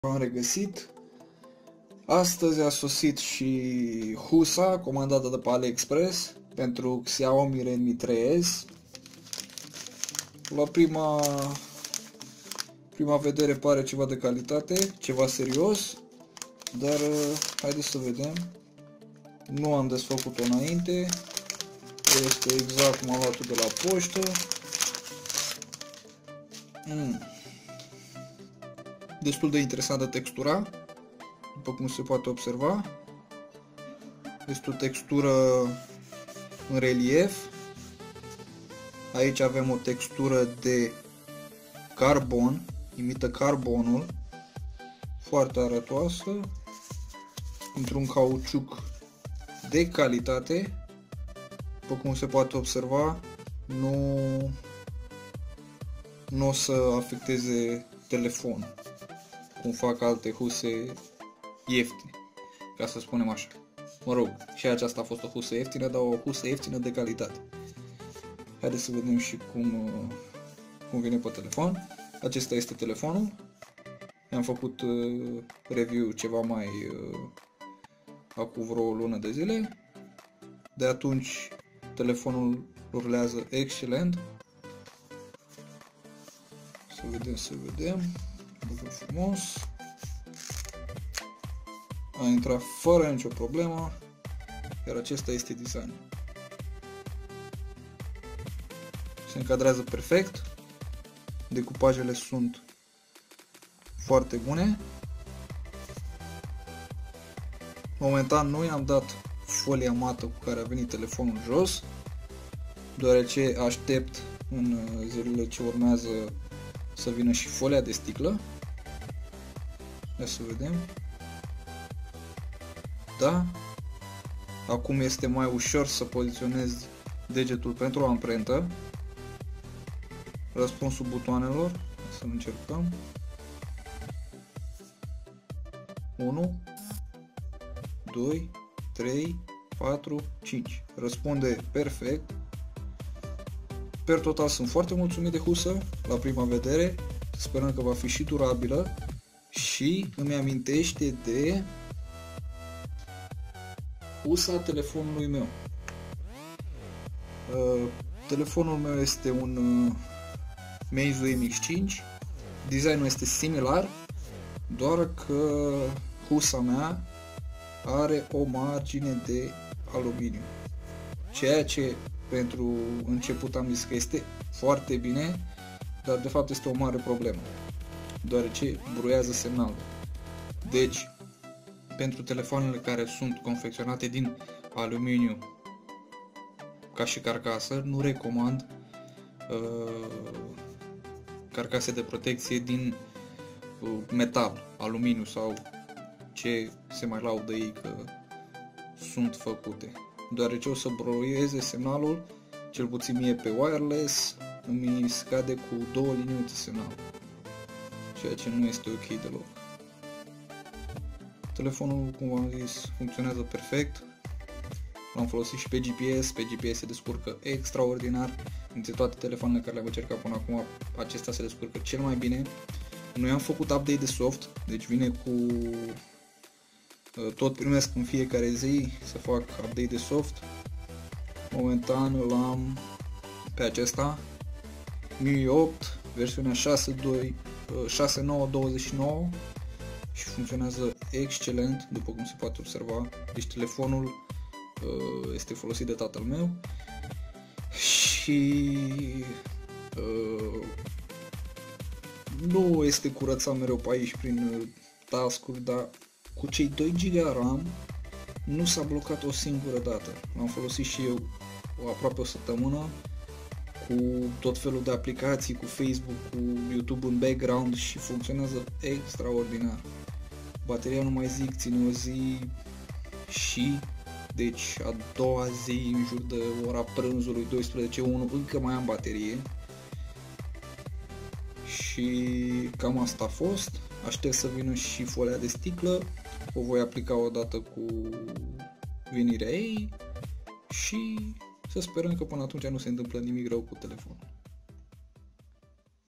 [HANDS-ON/UNBOXING] Xiaomi Redmi 3S Best Case
Husa este de foarte buna calitate si se muleaza perfect pe telefon. De asemenea, aceasta ii confera un design interesant si o aderenta mult mai buna in mana, astfel, riscul de a-l scapa pe jos scade considerabil.
Personal recomand acest produs datorita raportului calitate/pret imbatabil!